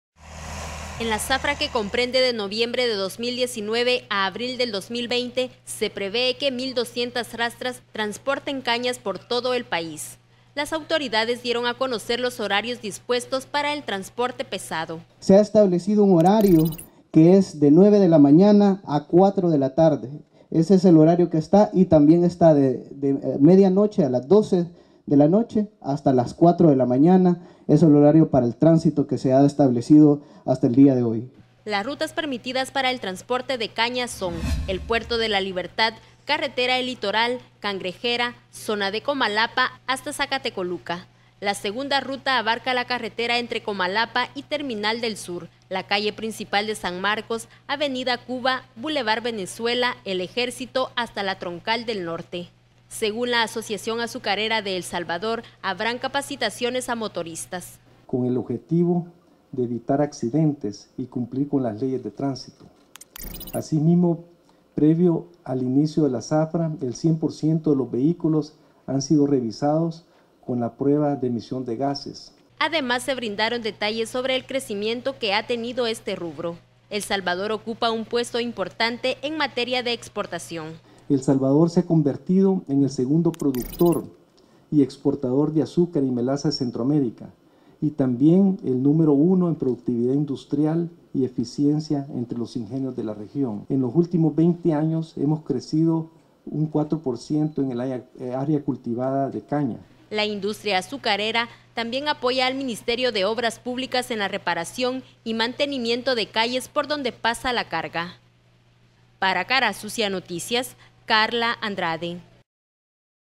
Autoridades anuncian horarios para el transporte pesado que trasladara caña de azúcar
Los horarios se han establecido con el objetivo de evitar accidentes de transito